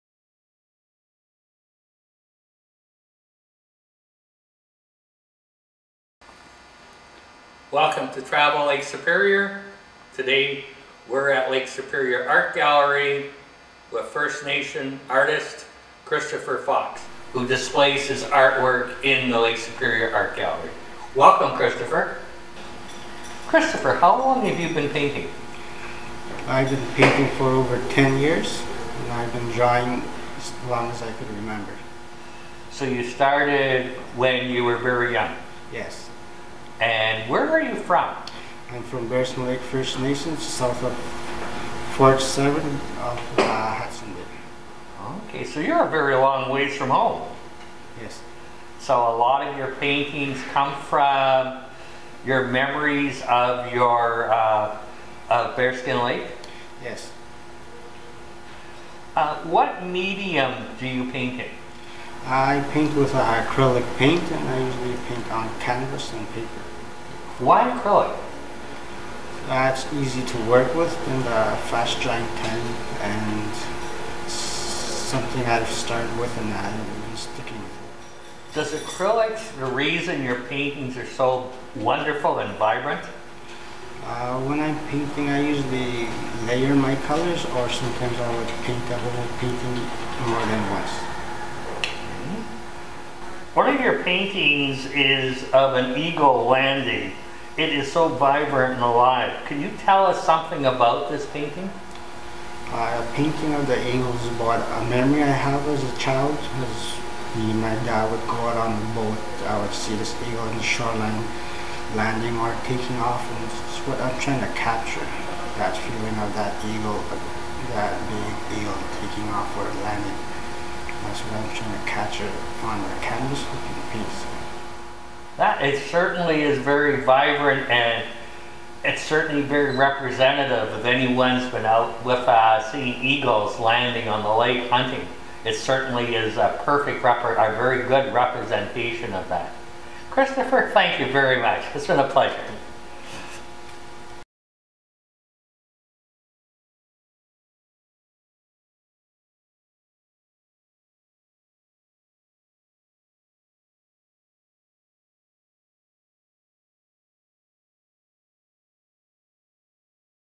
Artist Christopher Fox ~ Lake Superior Art Gallery
First Nation, Artist Christopher Fox is interview by JP Fraser at  Lake Superior Art Gallery about his Woodland Art.